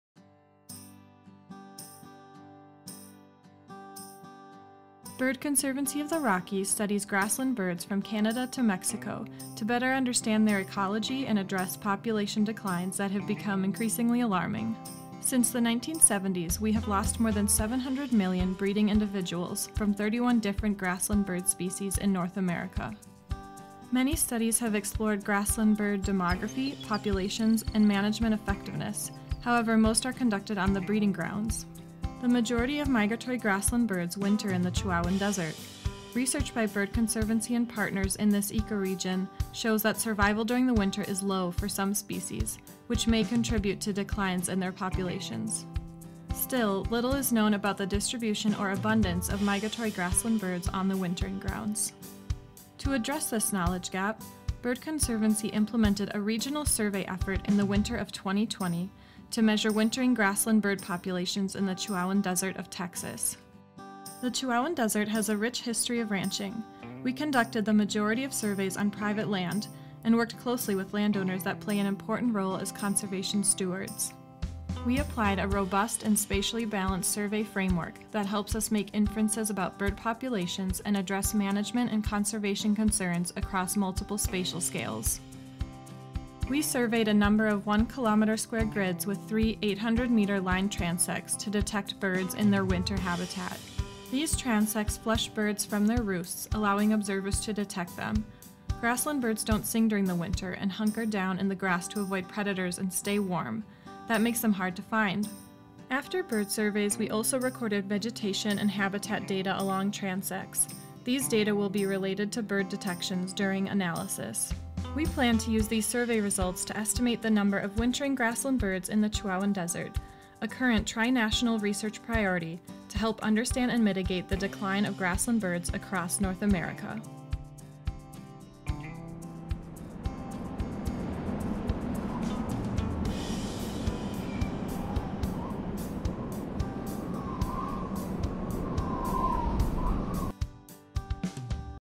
Winter Monitoring of Grassland Birds of the Trans-Pecos
Grassland bird populations are declining and the majority of species are understudied on their wintering grounds. In the winter of 2020, we implemented a regional monitoring program in the Chihuahuan Desert of Texas to establish baseline population estimates of grassland birds. We surveyed on a number of expansive cattle ranches, each exhibiting fascinating ecological and management histories. Through the implementation of this program, we can share that collaboration between ranching operations and grassland bird conservation is mutually beneficial.